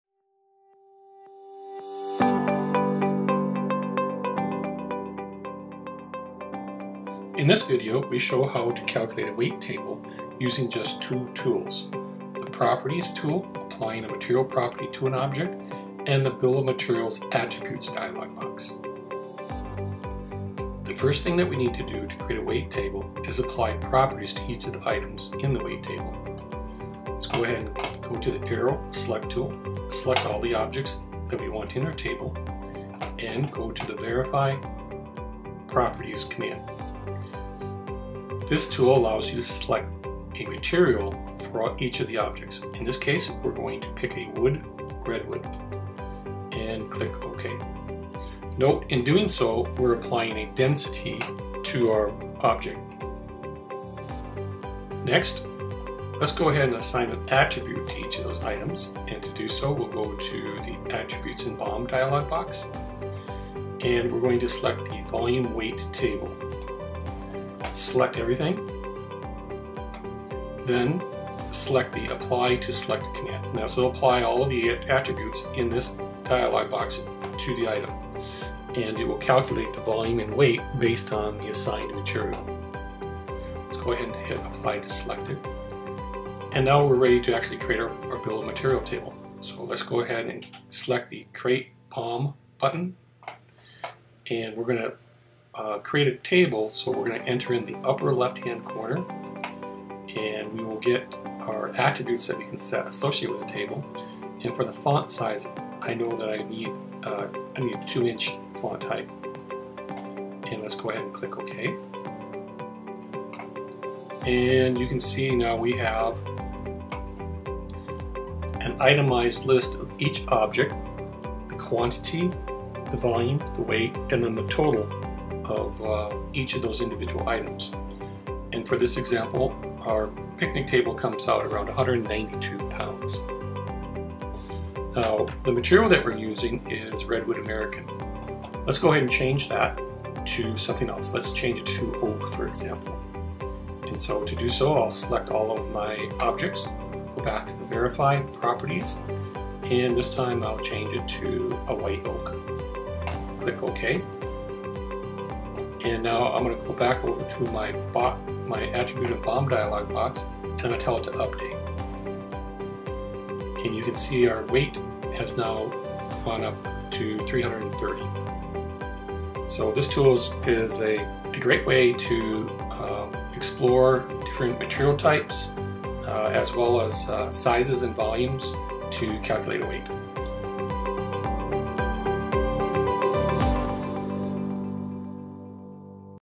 Punch!CAD v12 Tutorials- Weight and Volume Tables
Whether you’re designing an aircraft, automobile, or picnic table, weight is key control parameter in a design. V12 introduces a new Bill of Material(BOM) attribute specific for creating a summed weight table. Creating a weight table is based on the designer assigning a material using the Verify : Properties dialog box followed up by applying a weight attribute. With just these two steps, designers can create an accurate Weight table used to explore impacts of material types on design weights.